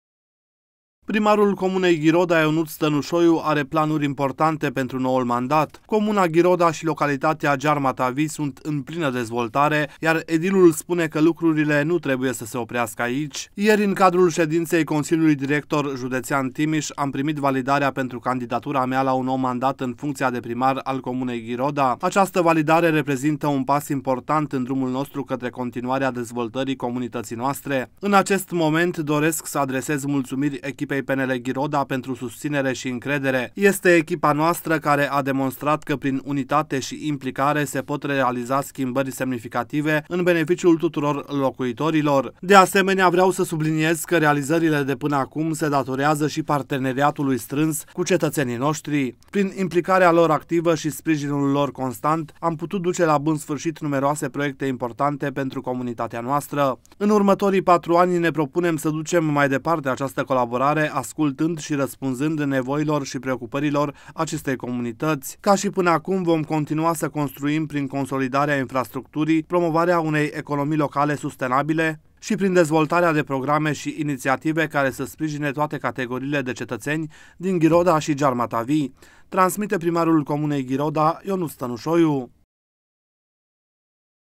PRIMARUL COMUNEI GHIRODA, IONUȚ STĂNUȘOIU, PLANURI IMPORTANTE PENTRU NOUL MANDAT
Primarul comunei Ghiroda, Ionuț Stănușoiu are planuri importante pentru noul mandat. Comuna Ghiroda și localitatea Giarmata Vii sunt în plină dezvoltare, iar edilul spune că lucrurile nu trebuie să se oprească aici. ”Ieri, în cadrul ședinței CDJ Timiș, am primit validarea pentru candidatura mea la un nou mandat în funcția de primar al comunei Ghiroda.